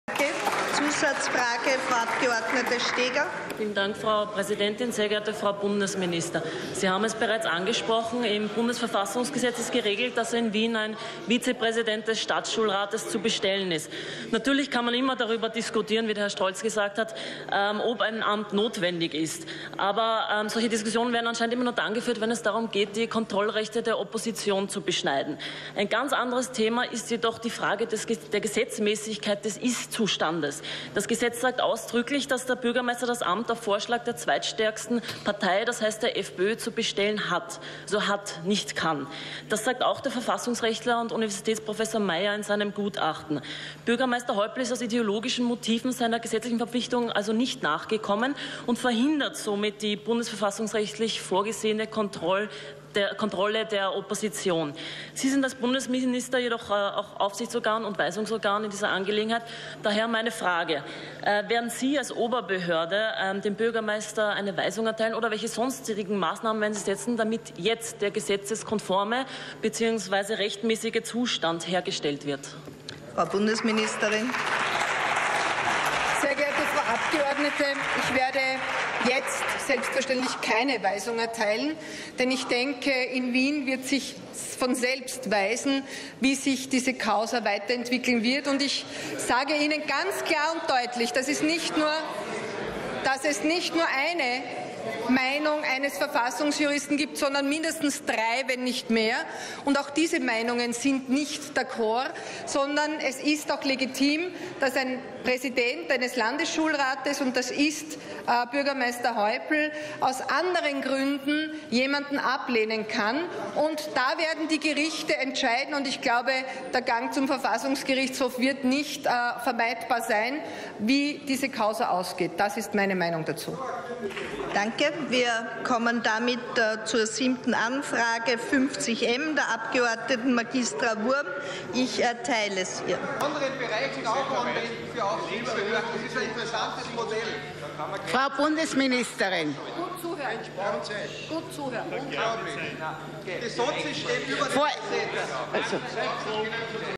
Petra Steger - Stadtschulrats-Vizepräsident - Weisungsrecht
FPÖ-Abgeordnete Petra Steger im Zuge der Fragestunde am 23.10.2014 über das Weisungsrecht der Bildungsministerin gegenüber dem Wiener Bürgermeister Häupl bei der Ernennung des vorgeschlagenen Vizepräsidenten des Wiener Stadtschulrates Maximilian Krauss.